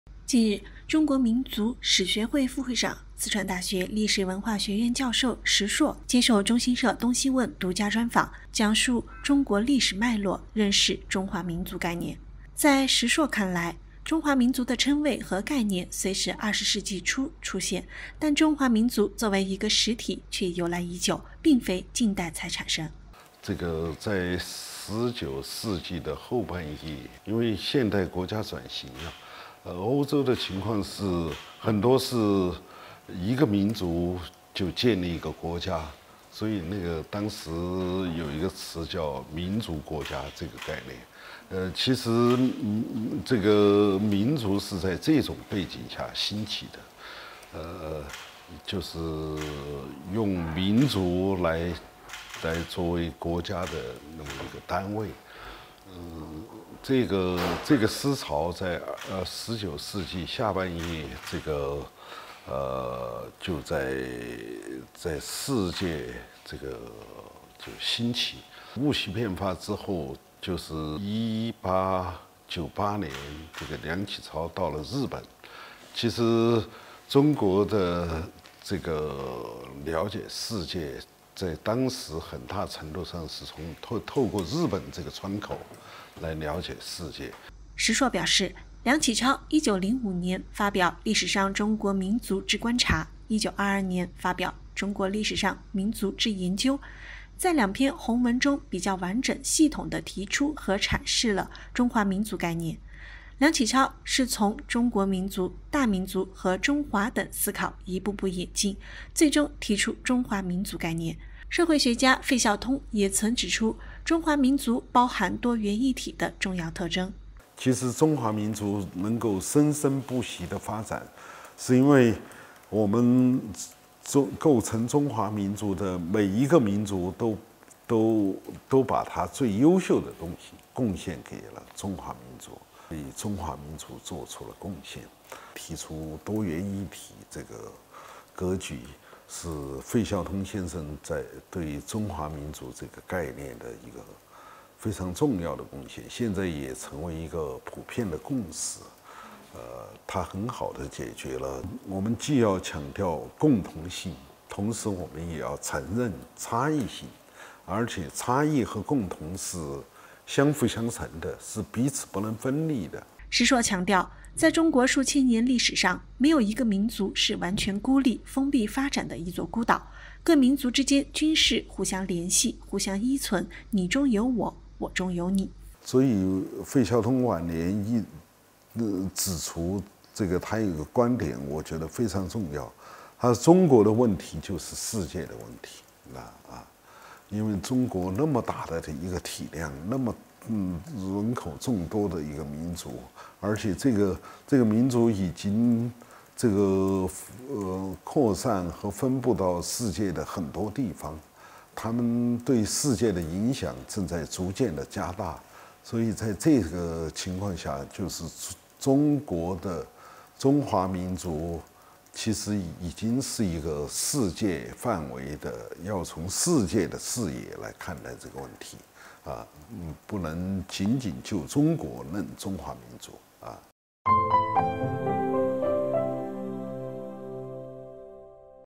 石硕： “中华民族”的概念到底从何而来？
近日，中国民族史学会副会长、四川大学历史文化学院教授石硕接受中新社“东西问”独家专访，讲述中国历史脉络、认识“中华民族”概念。
        在石硕看来，“中华民族”的称谓和概念虽是20世纪初出现，但中华民族作为一个实体却由来已久，并非近代才产生。